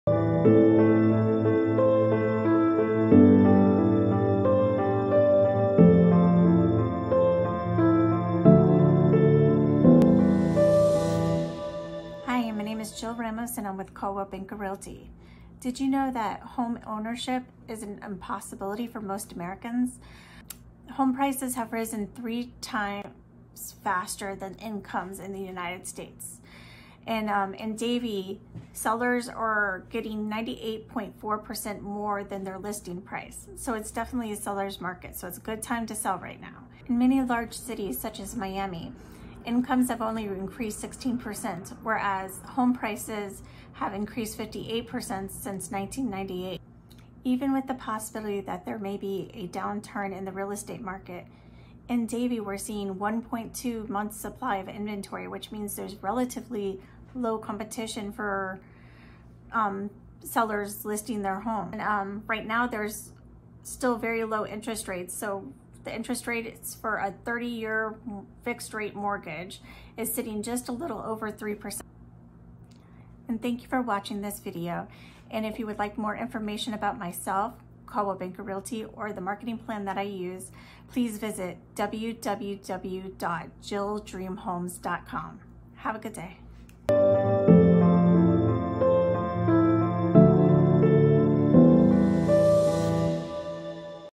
Davie Florida News: Davie Florida Real Estate Market Update November 2021
Real Estate Market Update for Davie Florida. What is going on with the Real Estate Market in Davie, Florida. How much is my home worth?